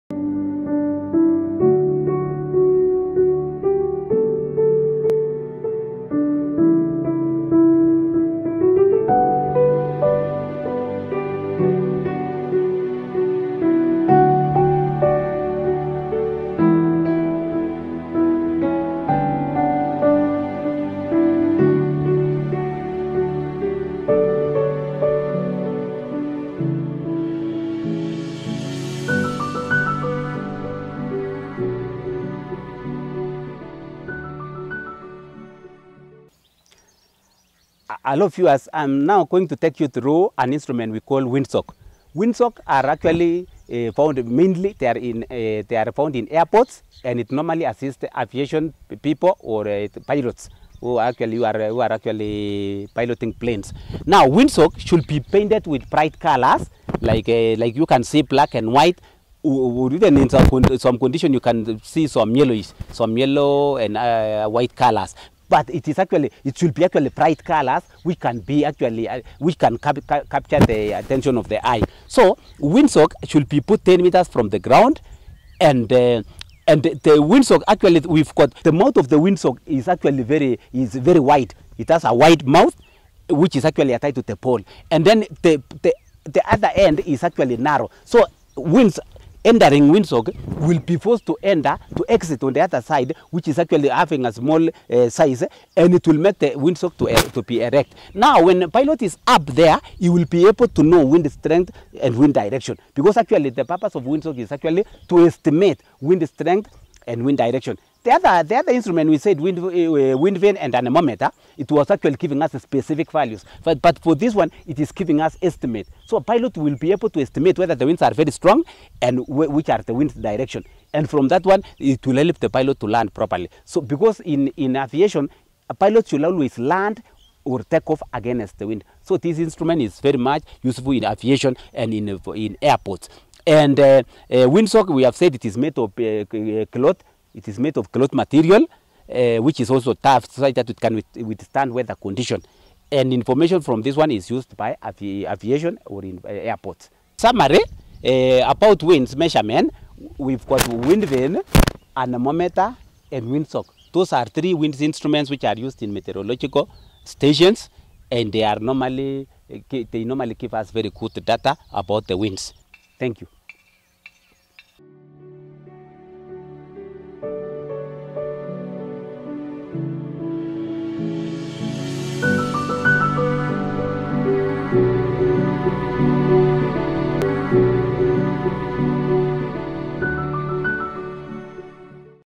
Windsock By Mr Mitei/Kongoi Weather Station Weather Instruments/Full HD Video 2020/ WindSock/Wind
Windsock is an instrument for measuring wind direction